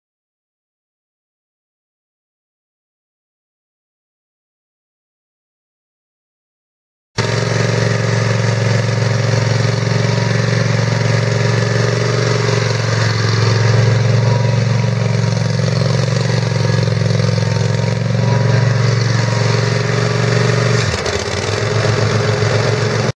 2008 Ferris Evolution lawn mower for sale | sold at auction June 11, 2014
2008 Ferris Evolution lawn mower
577 hours on meter
48" cut
Kawasaki 27 HP gas engine
Adjustable spring suspension seat and front wheels
Three blade deck
Pin hitch
Flat free front tires
20x10.00-10 rear tires
(2) sets of spare deck belts and blades included

It was located in Alma, KS, and we sold it to the highest bidder for $2,860.